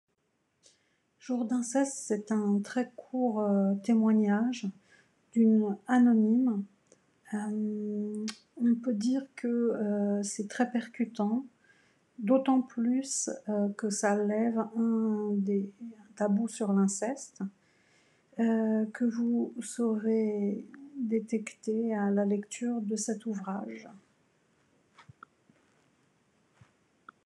Jours d'Inceste, Anonyme
Aux ÉDITIONS PAYOT & RIVAGES : les Inrocks le qualifie de "prétendu témoignage d'inceste". Ce n'est pas mon avis.